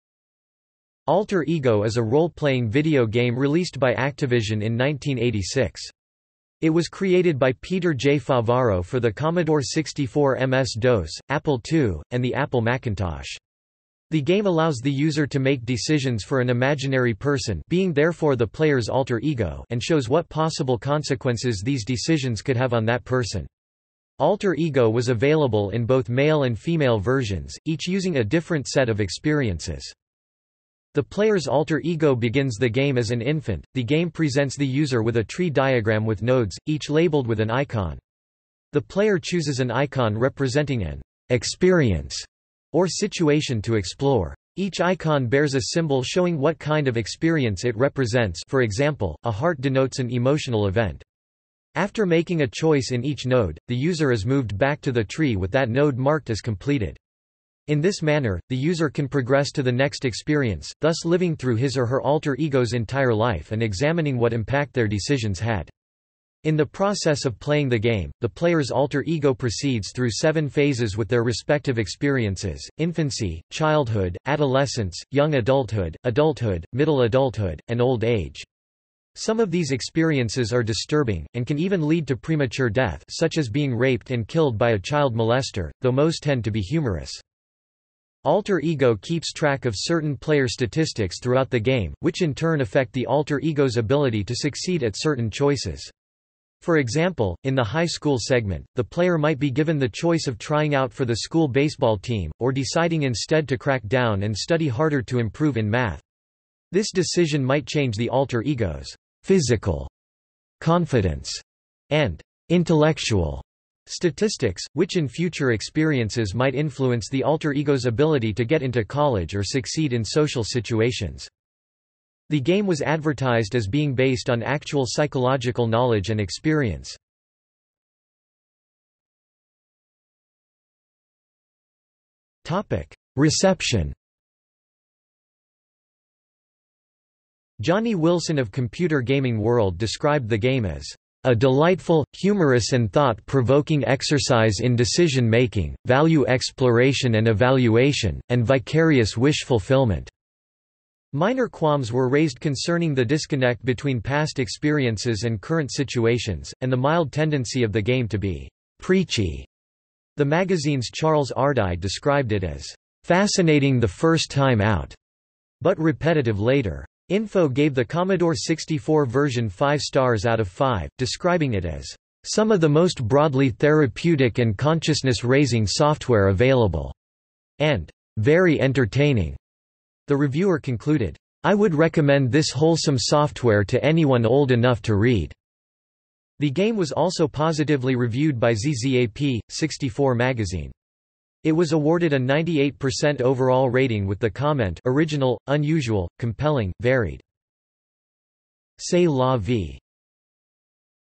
Alter Ego (game) | Wikipedia audio article
00:02:08 Reception

- Socrates

SUMMARY
Alter Ego is a role-playing video game released by Activision in 1986. It was created by Peter J. Favaro for the Commodore 64, MS-DOS, Apple II, and the Apple Macintosh. The game allows the user to make decisions for an imaginary person (being therefore the player's alter ego) and shows what possible consequences these decisions could have on that person. Alter Ego was available in both male and female versions, each using a different set of experiences.
The player's alter ego begins the game as an infant; the game presents the user with a tree diagram with nodes, each labeled with an icon. The player chooses an icon representing an "experience" or situation to explore. Each icon bears a symbol showing what kind of experience it represents (for example, a heart denotes an emotional event). After making a choice in each node, the user is moved back to the tree with that node marked as completed. In this manner, the user can progress to the next experience, thus living through his or her alter ego's entire life and examining what impact their decisions had. In the process of playing the game, the player's alter ego proceeds through 7 phases with their respective experiences: infancy, childhood, adolescence, young adulthood, adulthood, middle adulthood, and old age. Some of these experiences are disturbing, and can even lead to premature death (such as being raped and killed by a child molester), though most tend to be humorous.
Alter Ego keeps track of certain player statistics throughout the game, which in turn affect the alter ego's ability to succeed at certain choices. For example, in the high school segment, the player might be given the choice of trying out for the school baseball team, or deciding instead to crack down and study harder to improve in math. This decision might change the alter ego's "Physical", "Confidence", and "Intellectual" statistics, which in future experiences might influence the alter ego's ability to get into college or succeed in social situations.
The game was advertised as being based on actual psychological knowledge and experience.